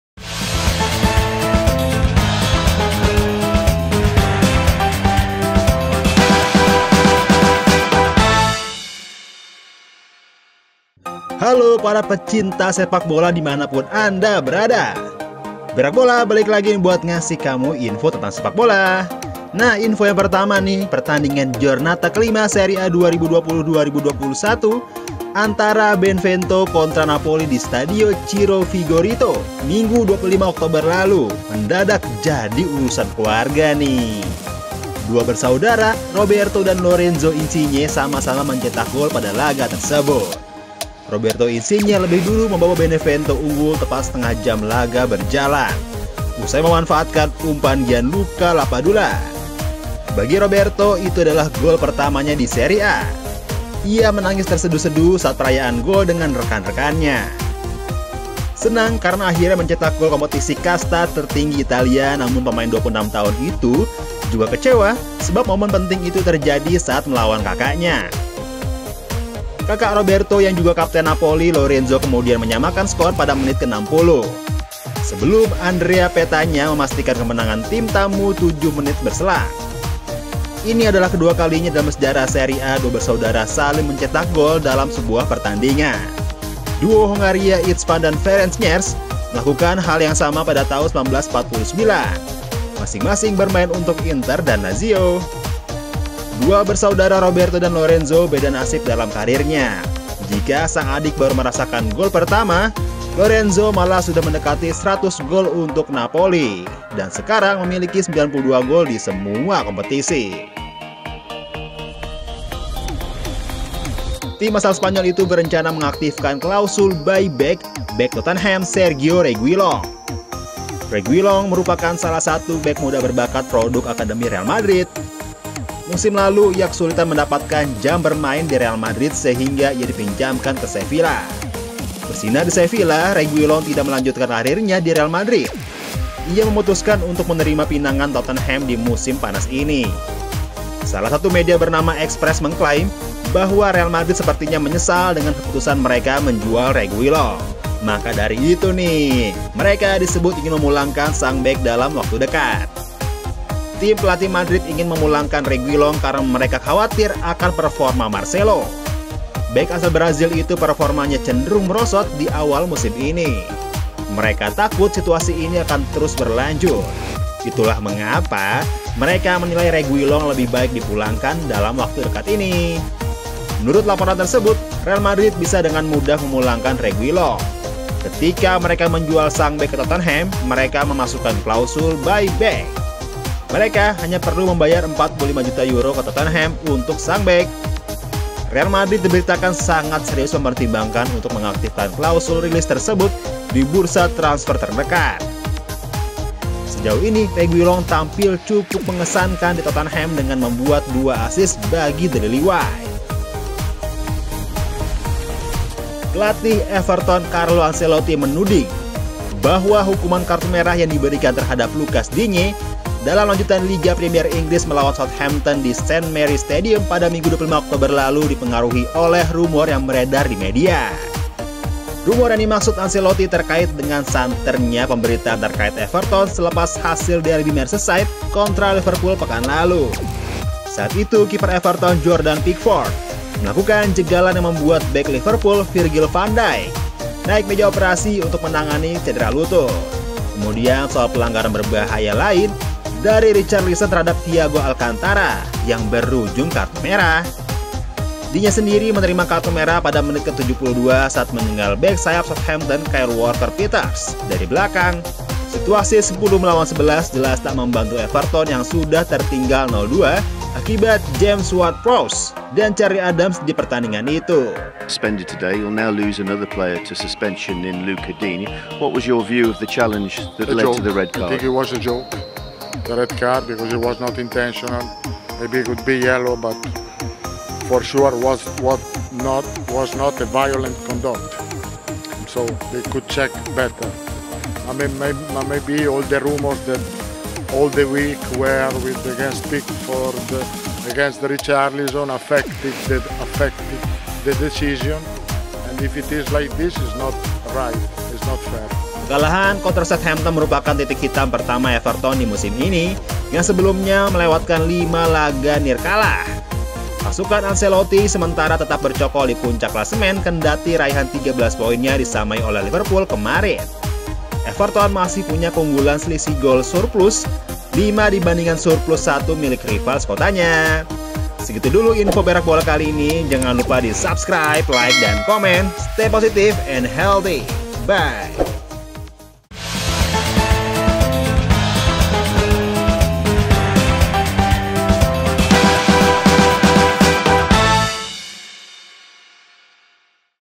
Real Madrid Ingin Memulangkan Reguilon, Ancelotti Ga Terima Kartu Merah Everton
Berak Bola dan ZQTV akan selalu memberikan informasi terkini seputar sepakbola

Follow Us on :
Google Play Store : ZQTV